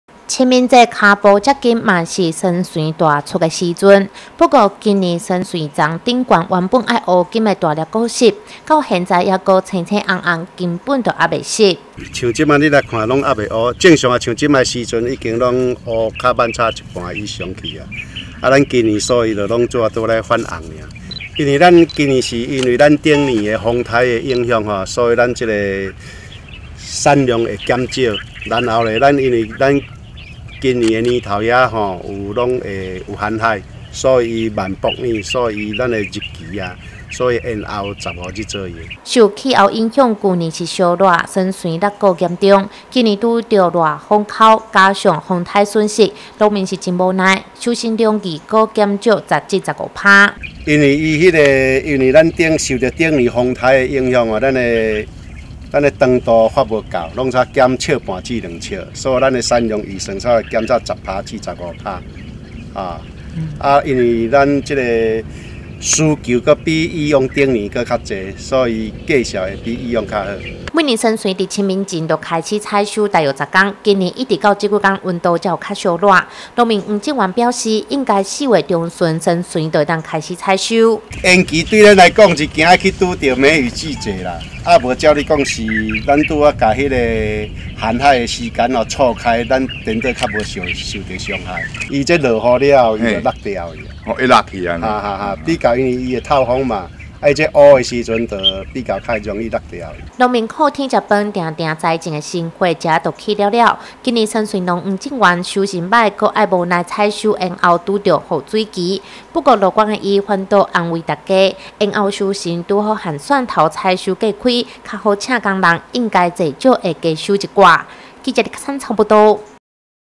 雲林新聞網-水林溫差大桑椹延後欠收
隨著夏天腳步接近，來一杯桑椹汁最解熱透心涼，不過今年因颱風影響，加上持續低溫，讓桑椹到現在還無法採收。在水林栽種桑椹有二十多年經驗的黃正源就表示，從來不曾遇到這樣的情形，以往這時候早就已到採收尾聲，以目前氣候狀況，今年桑椹可能得清明過後，才會達到成熟採收。